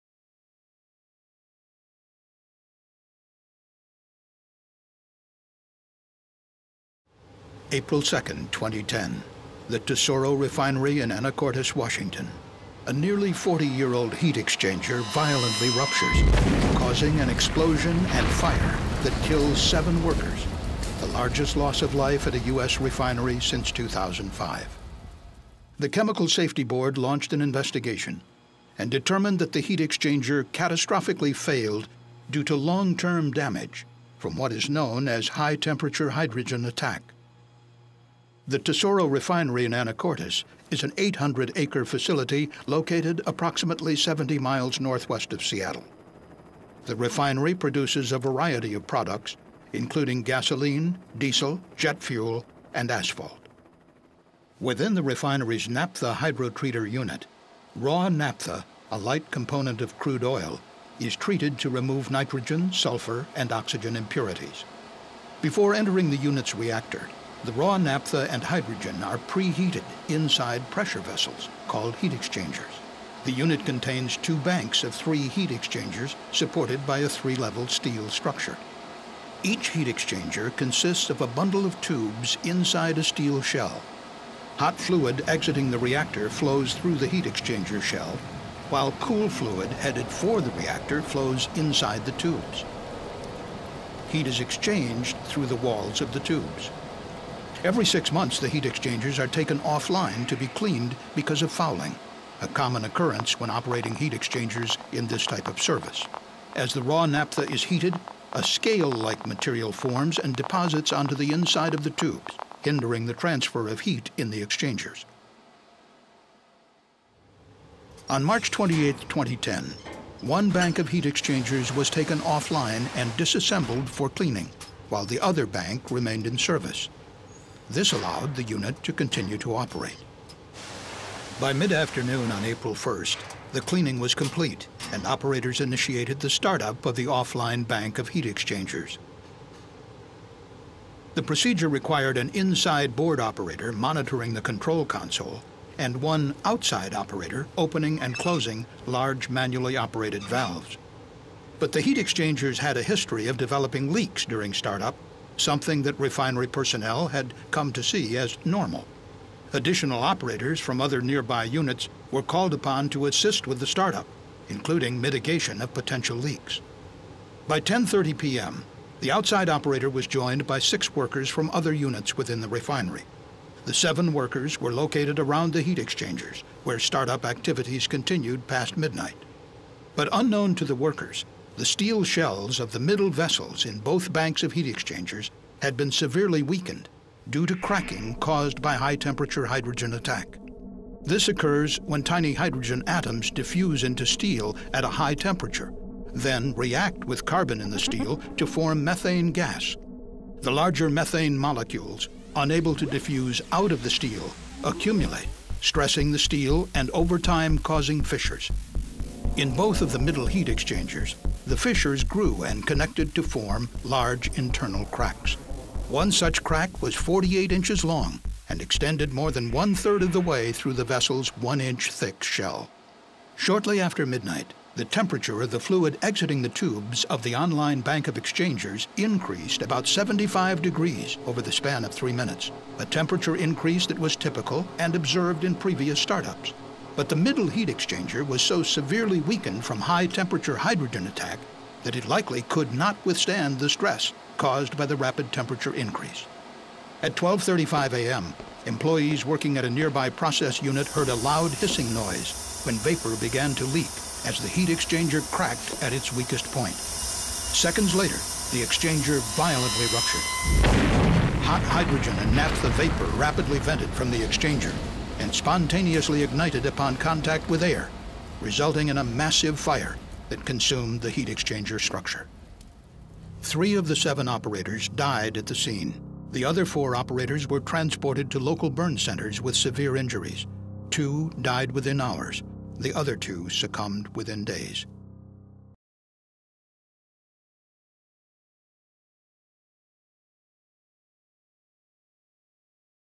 Animation of Explosion at Tesoro's Anacortes Refinery
An explosion and fire led to the fatal injury of seven employees when a nearly forty-year-old heat exchanger catastrophically failed on April 2, 2010.